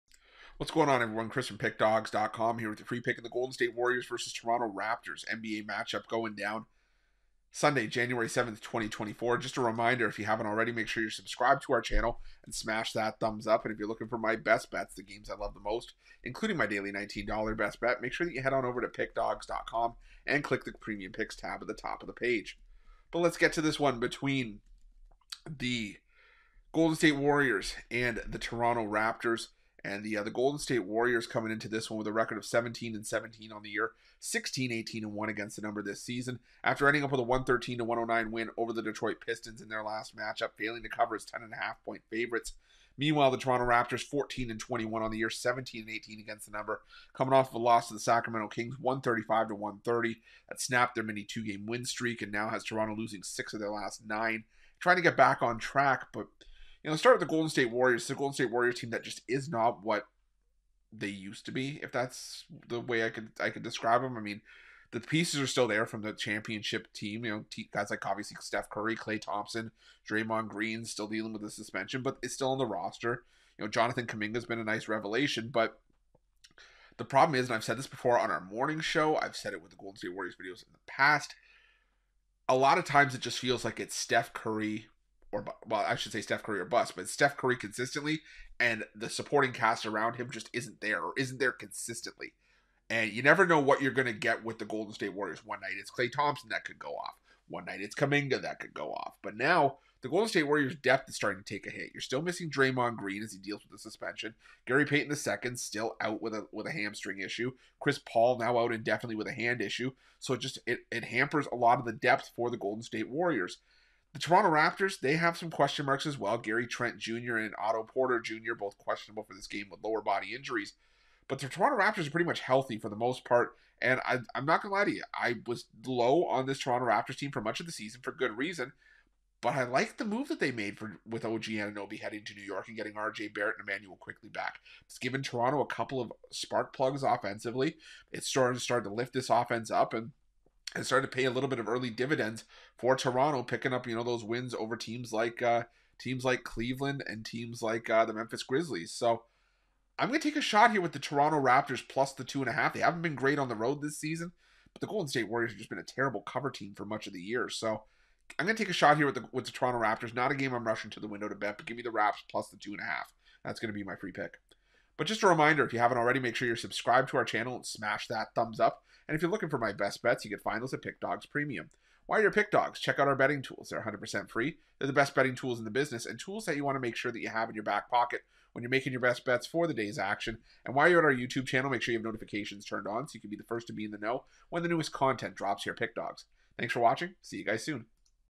Golden State Warriors vs Toronto Raptors 1/7/24 NBA Free Picks & Predictions | NBA Betting Tips
The Toronto Raptors face the Golden State Warriors in Sunday's NBA Basketball action. Get Chris' NBA Pick and Prediction for this game.